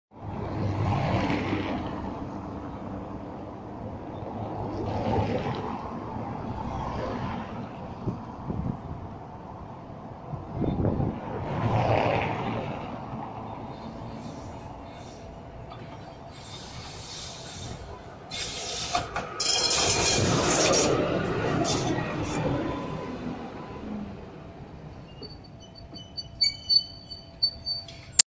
Sofia Tram Type T6M-700M #831 on line 4 at Krasno Selo ​
София Трамвай  T6M-700M # 831 на линия 4 в Красно Село